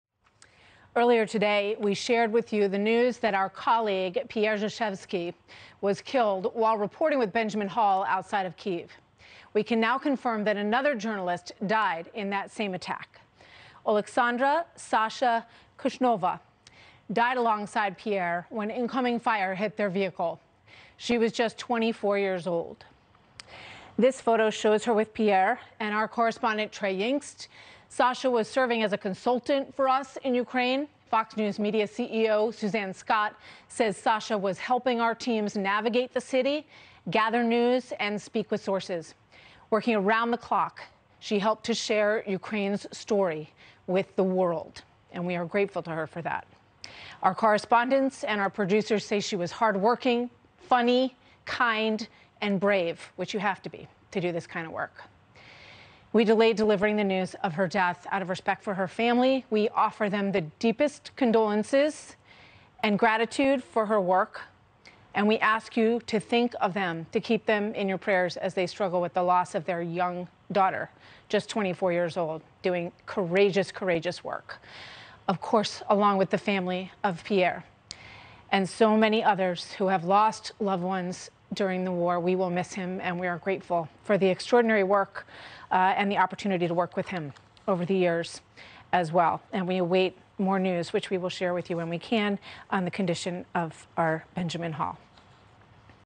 Journalist Oleksandra 'Sasha' Kuvshynova killed in Ukraine
Kuvshynova, who was serving as a Fox News consultant, was killed alongside cameraman Pierre Zakrzewski while in the field with Benjamin Hall.